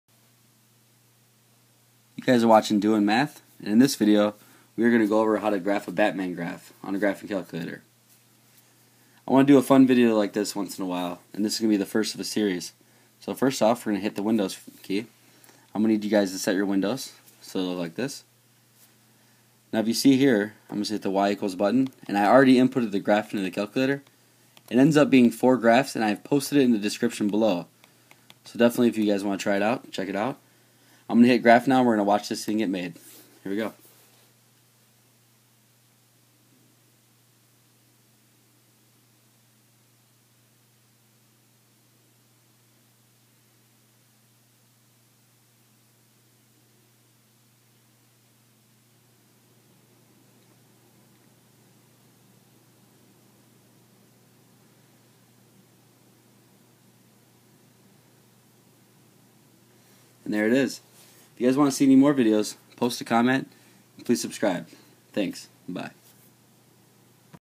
Batman Graph TI 84/83 Calculator
If you would like to see a specific video just post a comment
This is a graph i found on the internet and wanted to share it with you guys.

I would suggest that if you guys are going to type the equations into the calculator to copy and paste equation into word because it will be a lot easier to read.

 Y1 = 1.5sqrt(-abs(abs(x)-1)*abs(3-abs(x))/((a¬bs(x)-1)*(3-abs(x))))(1+abs(abs(x)-3)/(a¬bs(x)-3))sqrt(1-(x/7)^2)+(4.5+0.75(abs(x¬-.5)+abs(x+.5))-2.75(abs(x-.75)+abs(x+.7¬5)))(1+abs(1-abs(x))/(1-abs(x)))

Y2 = -3sqrt(¬1-(x/7)^2)sqrt(abs(abs(x)-4)/(abs(x)-4))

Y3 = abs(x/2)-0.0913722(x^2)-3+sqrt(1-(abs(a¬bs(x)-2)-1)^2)

Y4 = (2.71052+(1.5-.5abs(x))-1¬.35526sqrt(4-(abs(x)-1)^2))sqrt(abs(abs(¬x)-1)/(abs(x)-1))